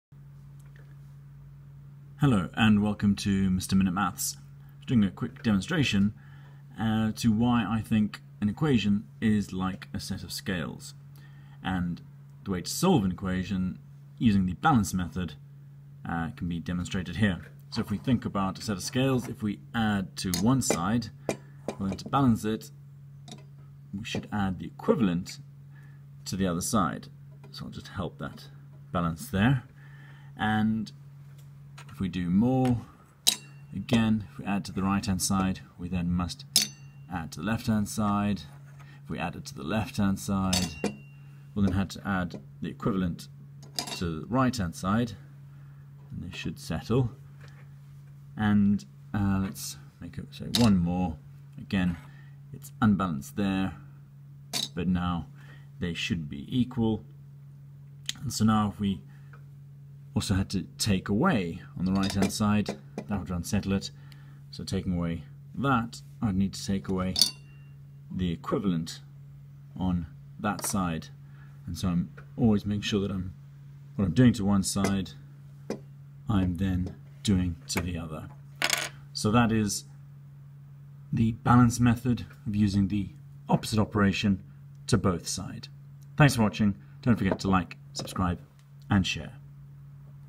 Maths equations balance method (demo with scales)
Using a set of scales and some Lego to demonstrate the balance method way of solving equations:
Opposite operation
To Both sides

"what you do to one side, you do to the other", hence the "balance"

Look out for more videos in the Lego and Shapes and exams playlists!

Good luck for your exams!

Email or tweet requests and suggestions